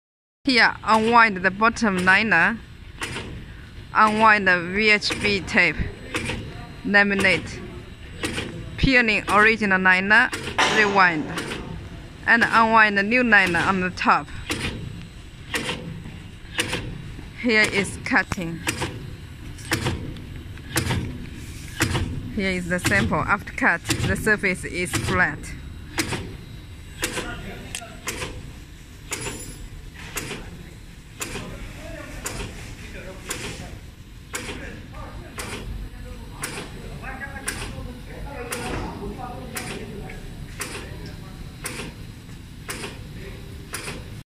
DTH-700 Laminating sheet cut VHB tape
Laminate sheet cut VHB tapes.
Before cut, change liner of the VHB tape.
2 laminate stations, helpful for make sure after laminate, final product is flat.
Popular for convert VHB tapes, and other foam tapes.

We keep machine with sensor, it is also ok work as roll to sheet cutting machine.

Chris
wechat: convertchris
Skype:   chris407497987